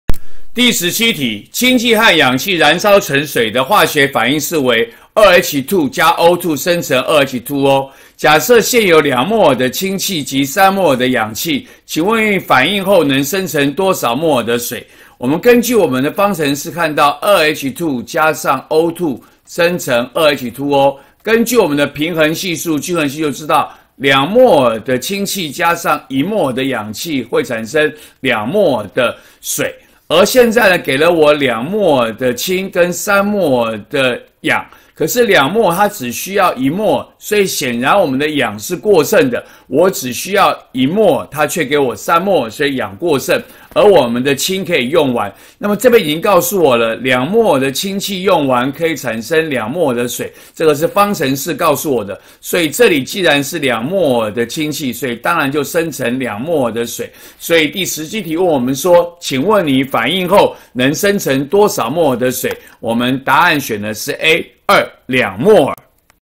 104學年下8年級理化一段第17題
給氫氣和氧氣反應方程式，及氫氣、氧氣莫耳數，問可生成水多少莫耳。